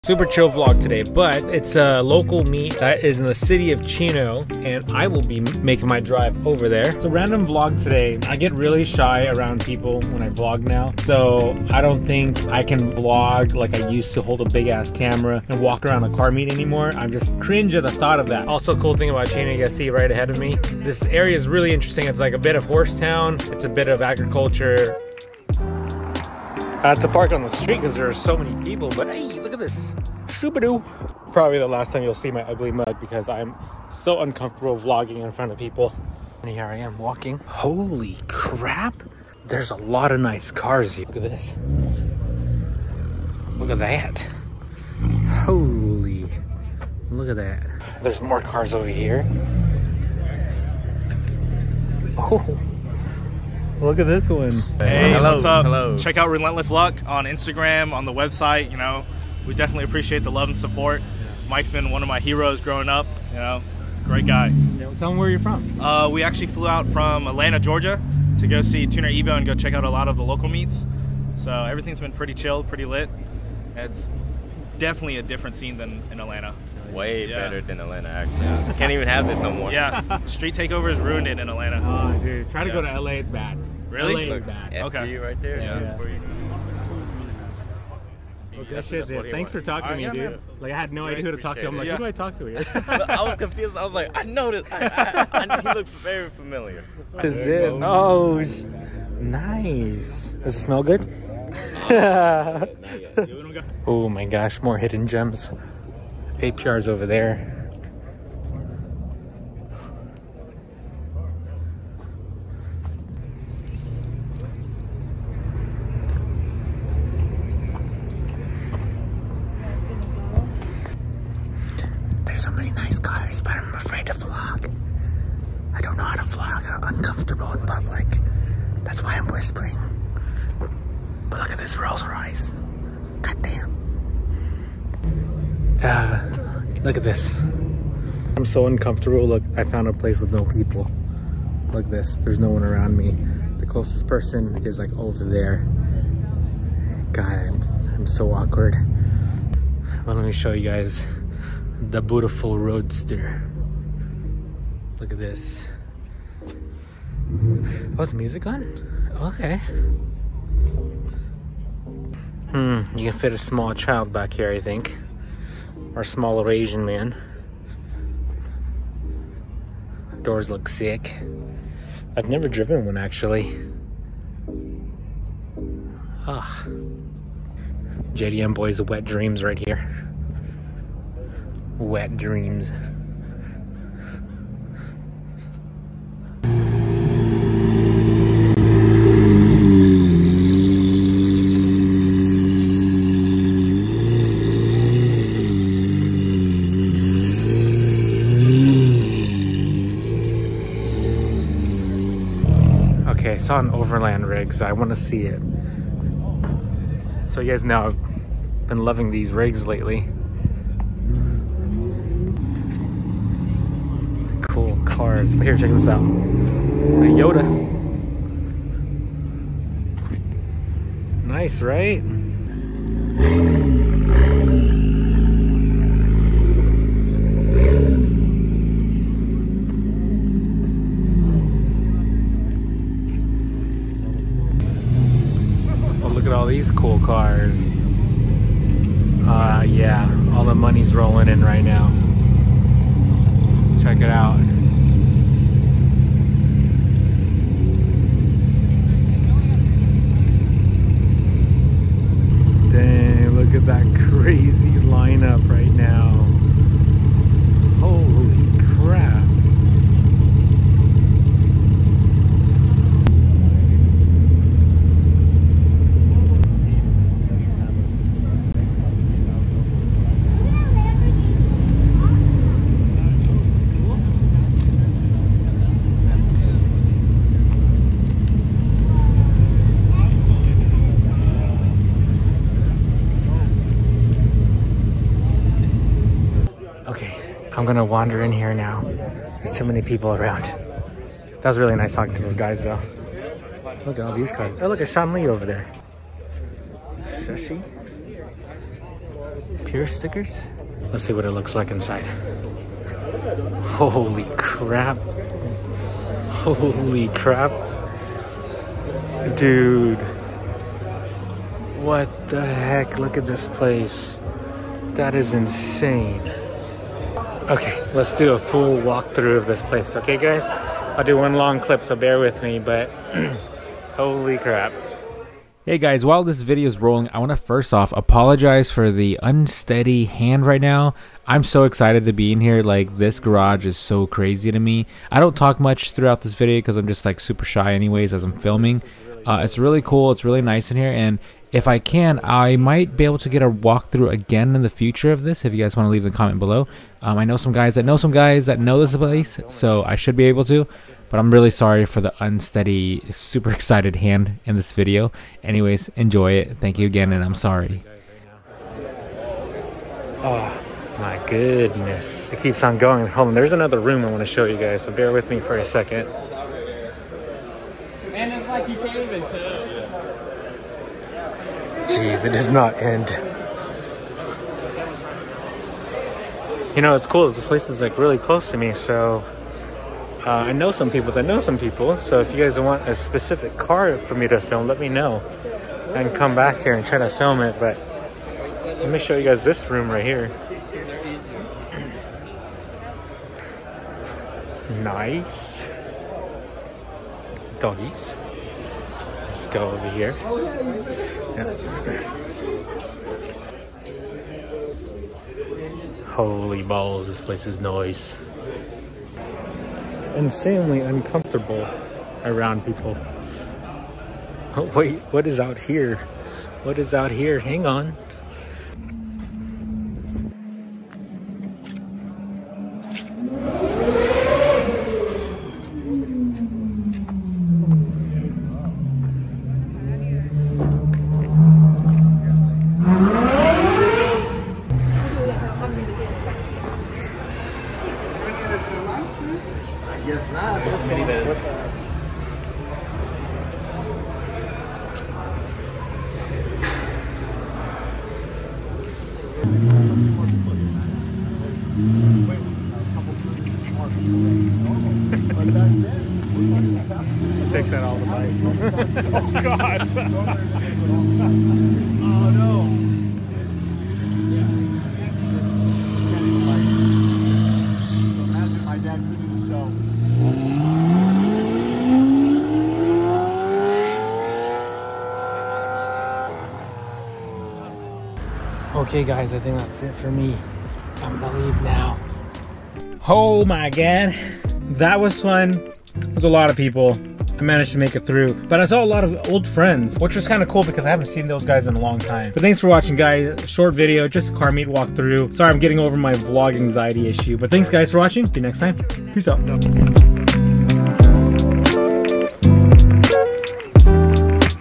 I FEEL UNCOMFORTABLE HERE | car meet vlogging
I went to the local WRTeknica car meet and saw some amazing cars but realized Im terrible at vlogging.

CAMERA EQUIPMENT LINKS:
Camera: Sony a6000
176 LED light panel
Tripod
►My Clothing Brand!

Mail Box Fan Mail
1155 S. Milliken Avenue
Suite D